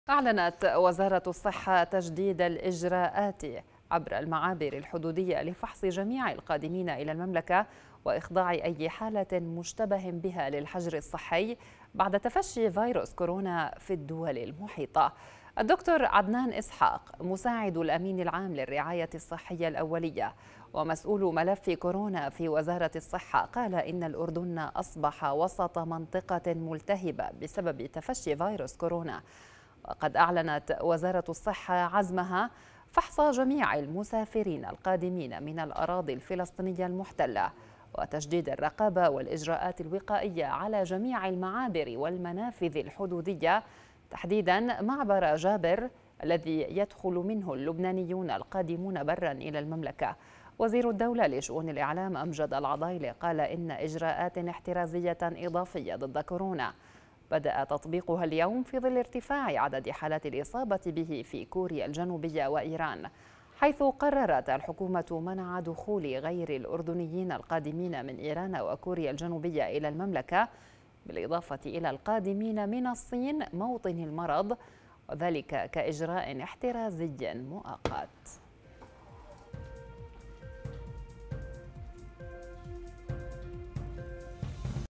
إجراءات احترازية مشددة بعد تسجيل إصابات بالكورونا في الدول المحيطة - (23/2/2020)
أعلنت وزارة الصحة تشديد الإجراءات عبر المعابر الحدودية لفحص جميع القادمين إلى المملكة وإخضاع أي حالة مشتبه بها للحجر الصحي، بعد تفشي فيروس كورونا في الدول المحيطة.